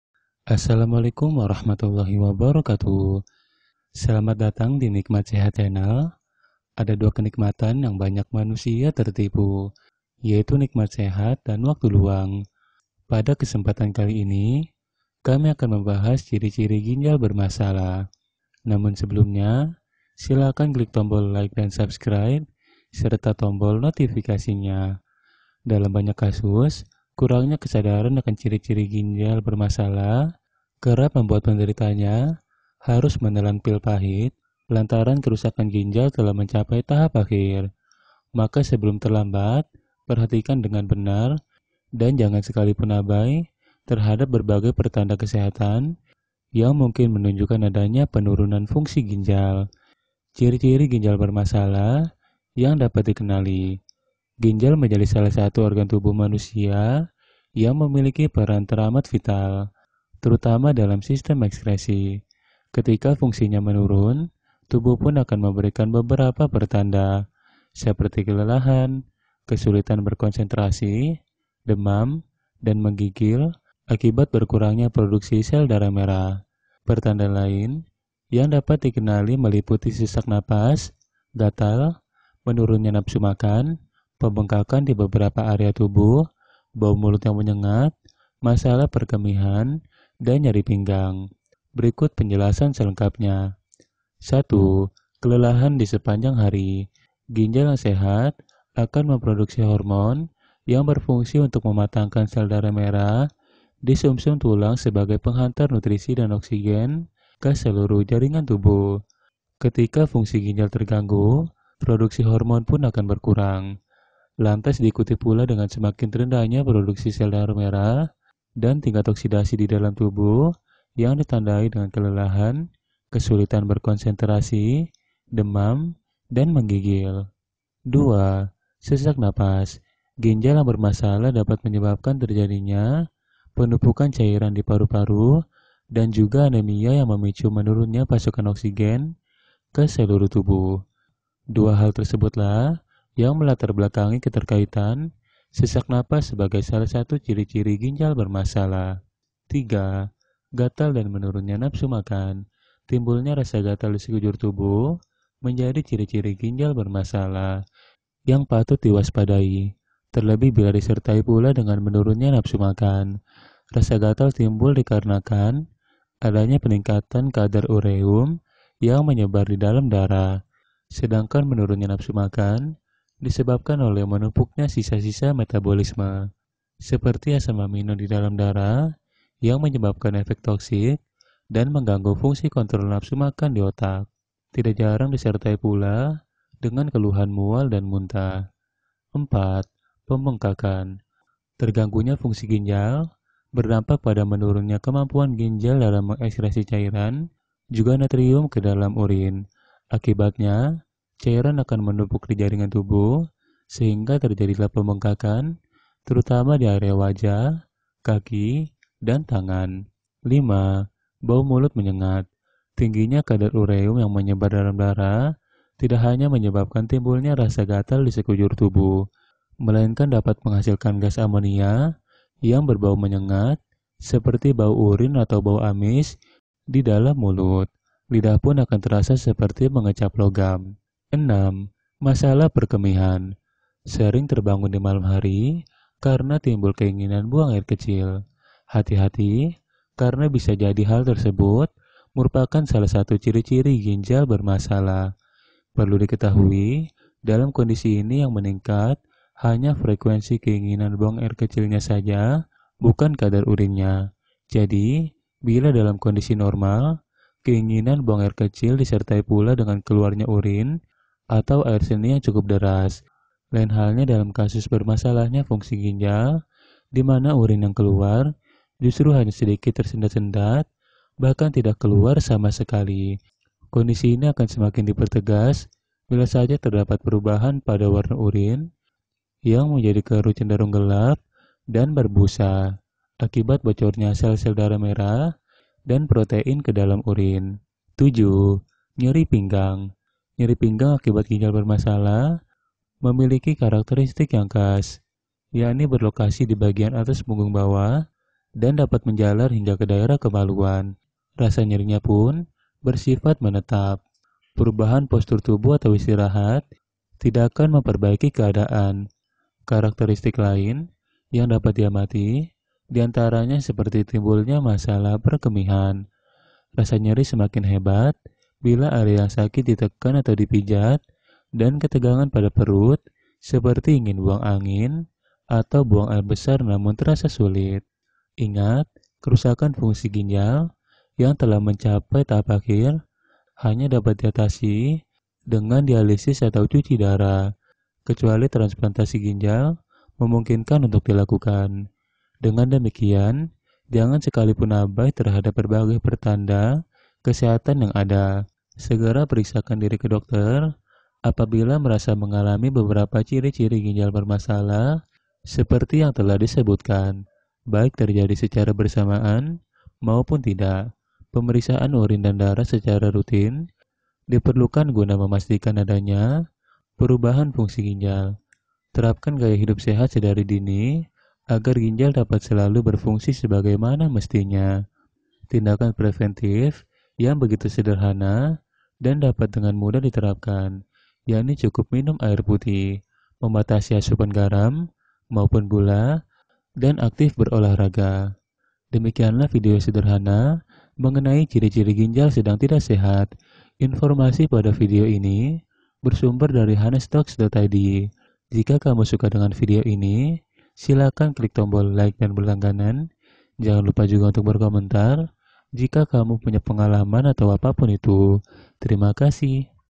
CIRI CIRI GINJAL KAMU SEDANG TIDAK SEHAT- 7 Ciri Ginjal Sedang Bermasalah
Dalam banyak kasus, kurangnya kesadaran akan ciri-ciri ginjal bermasalah kerap membuat penderitanya harus menelan pil pahit lantaran kerusakan ginjal telah mencapai tahap akhir. Maka sebelum terlambat, perhatikan dengan benar dan jangan sekalipun abai terhadap berbagai pertanda kesehatan yang mungkin menunjukkan adanya penurunan fungsi ginjal.